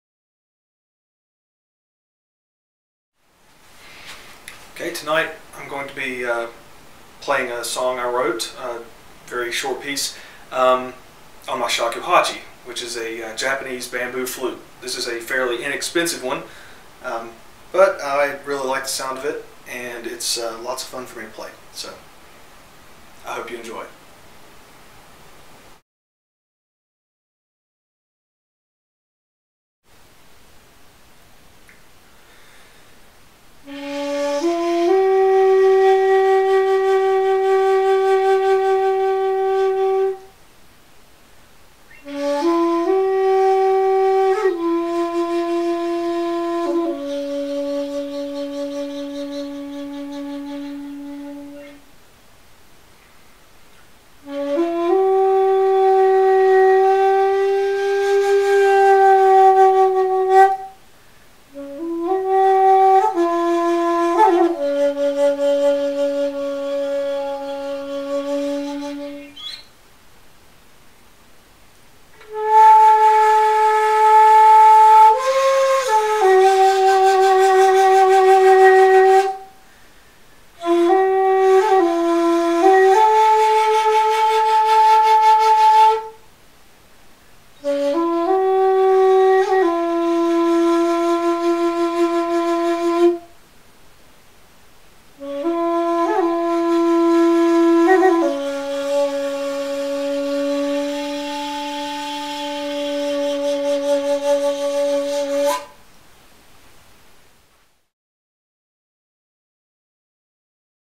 Month of Music - Day 2
For each day in the month of December, I plan to compose a unique, short tune and post it on online. This is the second one, which I wrote a couple of minutes before I performed it for this video.